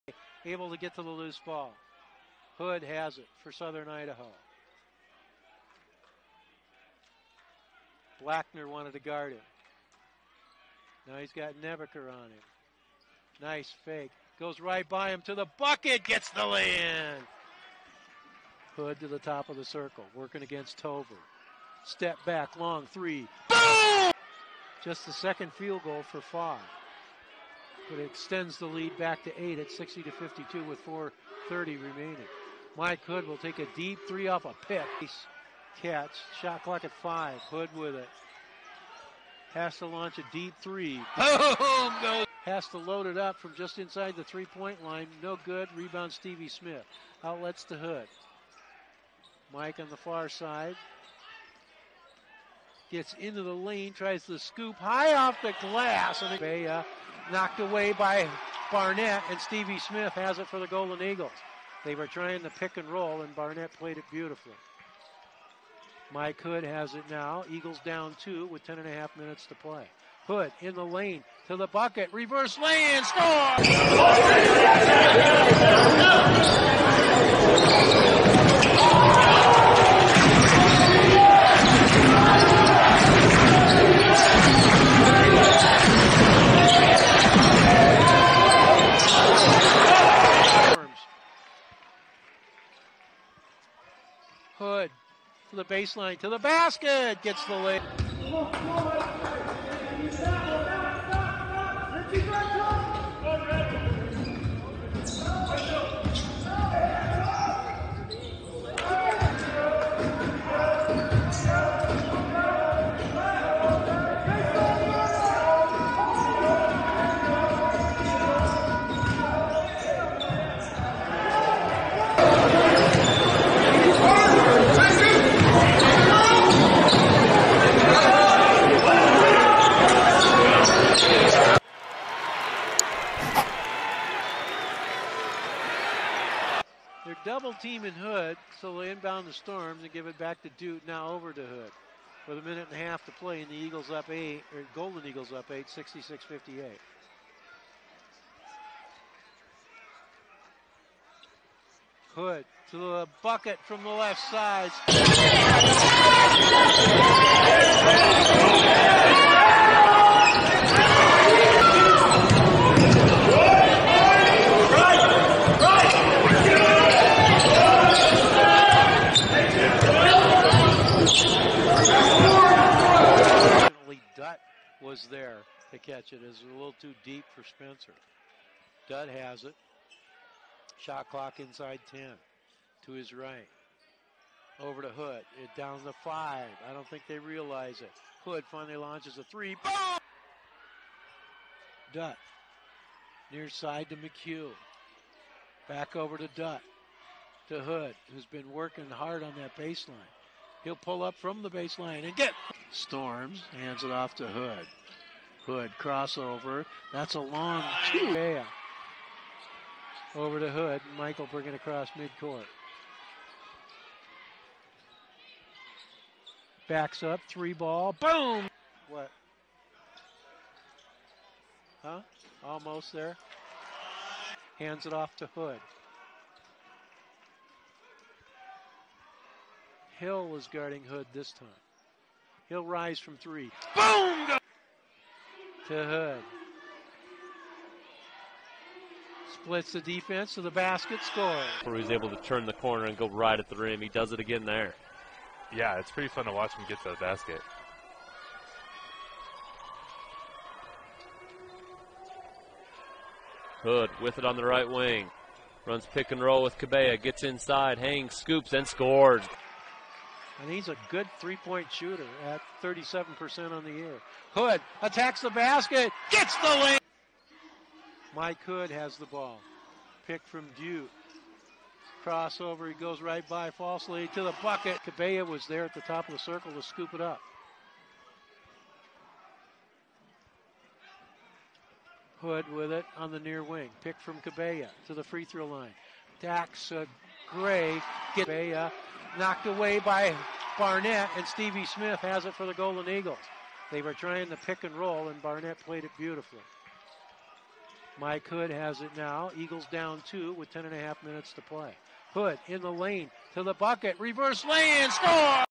Mike Hood College of Southern Idaho 2019-2020 Highlight Montage | NEWEST MONTANA STATE BOBCAT
Mike Hood capped off his NJCAA career by earning NJCAA Honorable All-American Mention. The guard, who will take his game to Montana State next year, etched his name in the CSI record books. Hood finished his stint at CSI as the the season record holder for most points in a season (795) and most points per game in a season (22.7 ppg). He also averaged 1.9 assists, 3.4 rebounds and 1.5 steals per game for the Golden Eagles. He knocked down an incredible 87 percent of his free-throw shots as well. Hood earned Scenic West Athletic Conference Player of the Week twice during the season. He also earned a spot on the Regional 18 first team during his short run with the Golden Eagles. For more exclusive content, don't forget to click the link to our website.